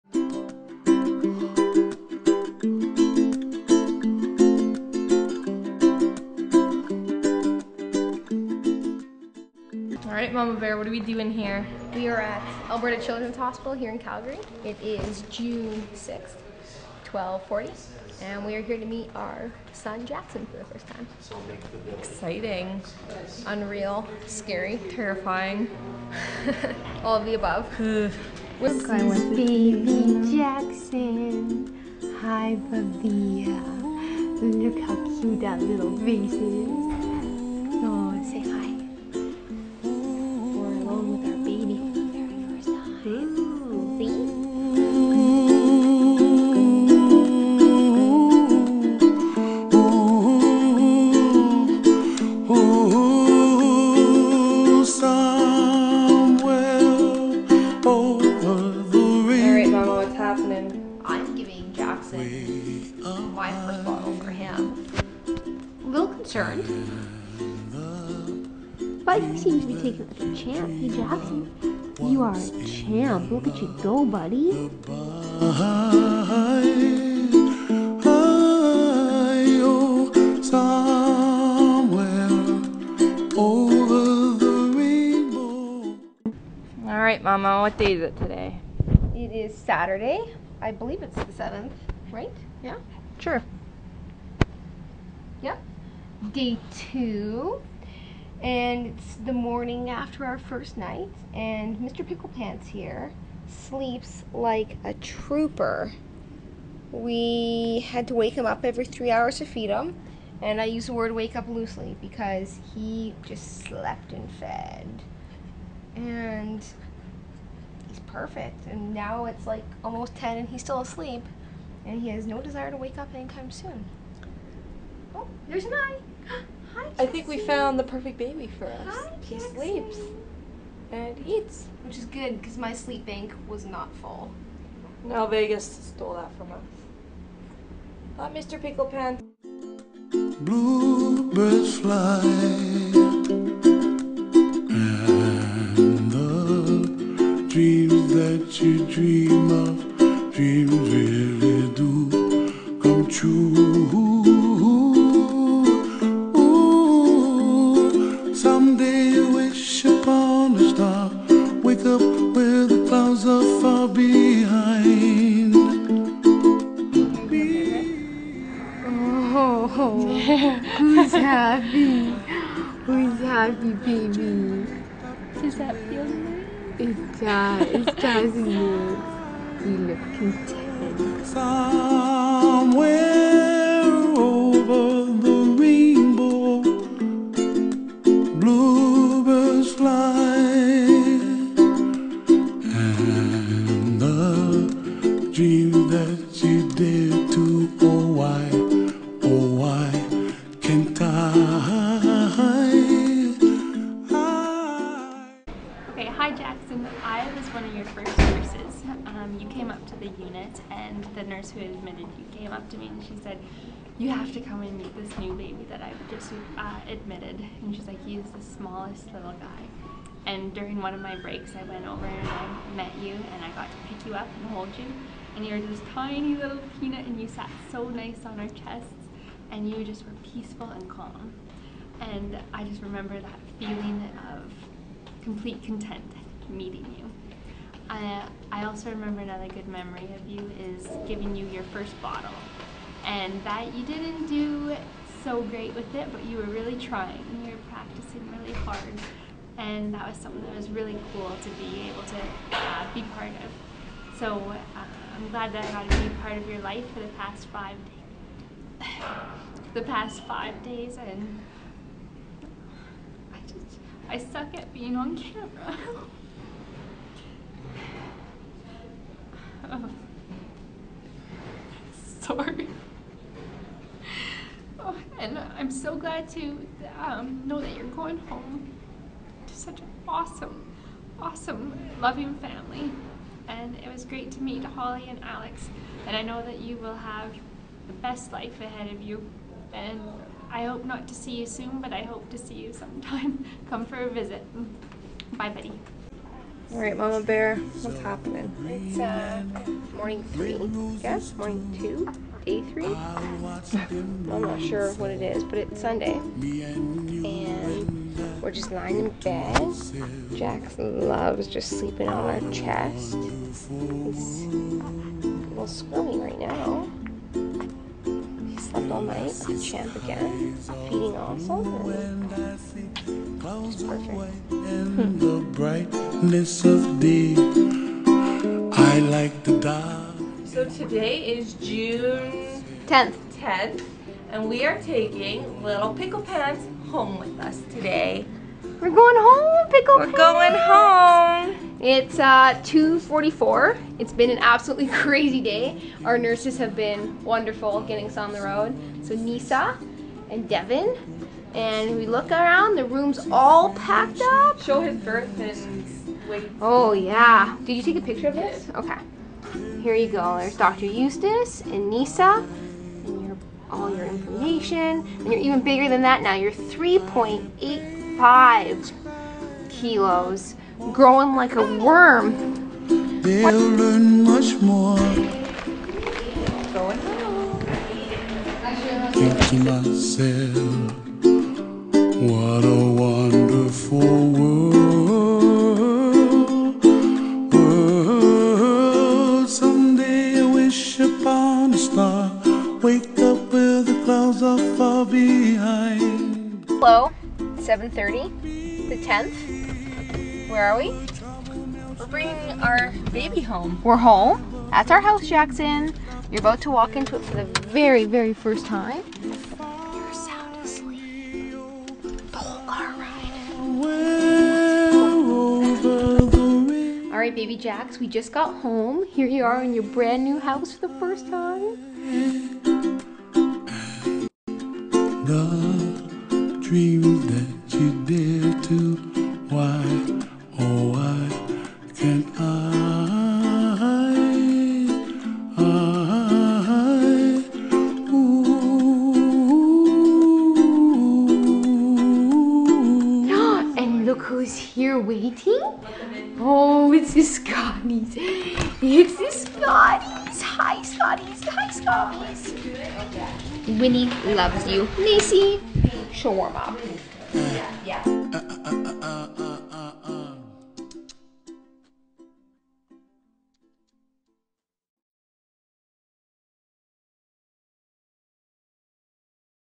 Welcome Home - A Down Syndrome Adoption Story
Our insanely quick adoption journey of our perfect son born with Down syndrome